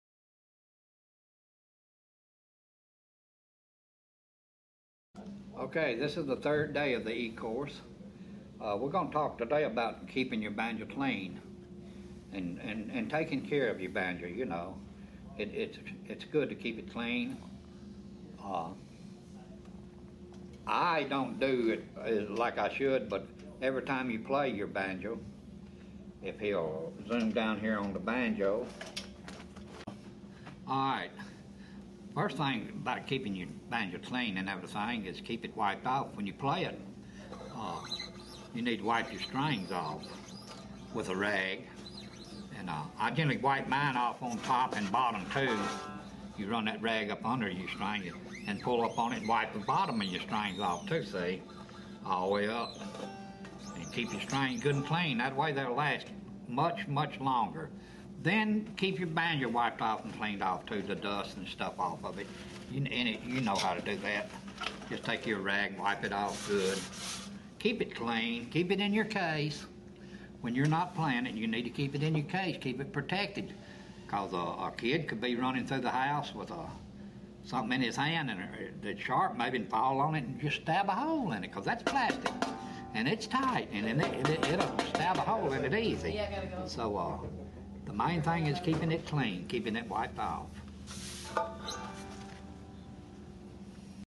Cleaning your Banjo: Day Three
This is day three of our free ecourse.  We tell about cleaning your banjo.  It can help the sound and everything else.